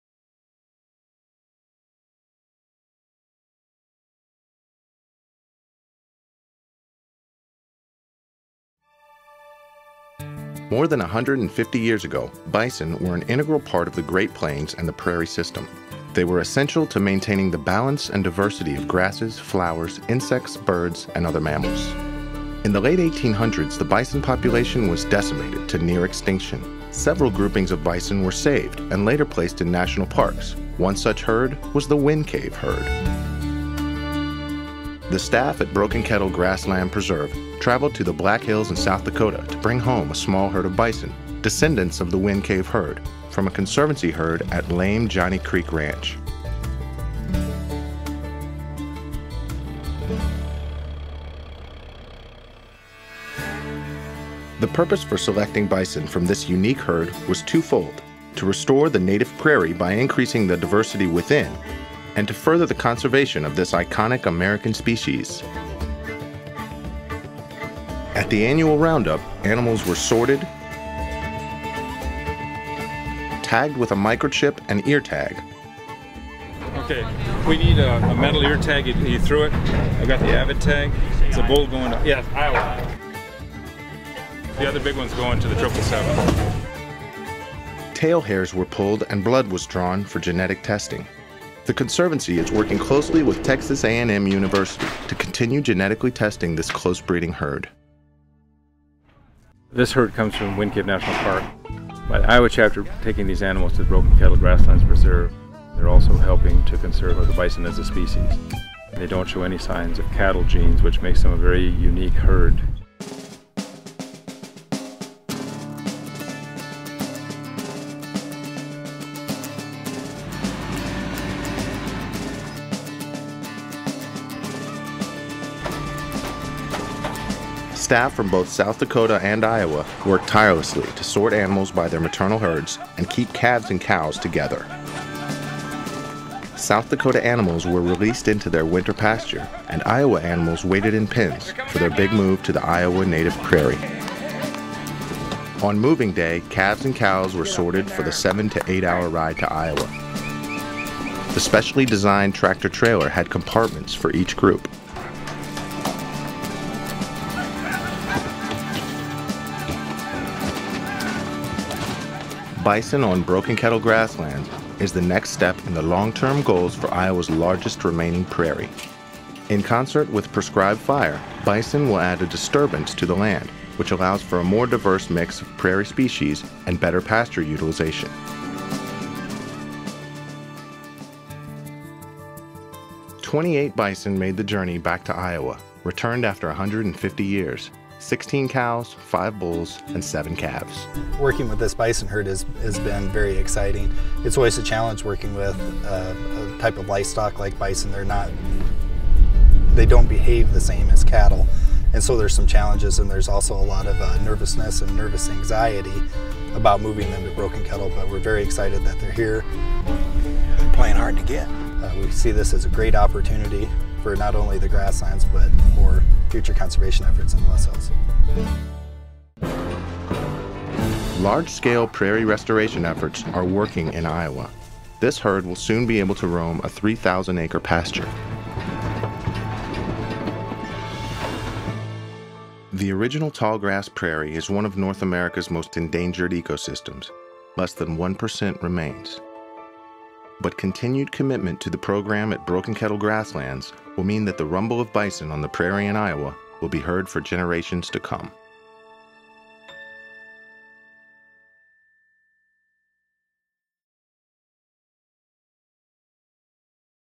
Bison at Broken Kettle Grasslands
At the end of October 2008, a small herd of bison will be reintroduced at Broken Kettle Grasslands in the globally significant Loess Hills, in western Iowa. These bison are part of the prairie restoration effort that has diversified that plant and animal communities in the stunningly beautiful landscape. Learn more about bison: Why now? Why bison?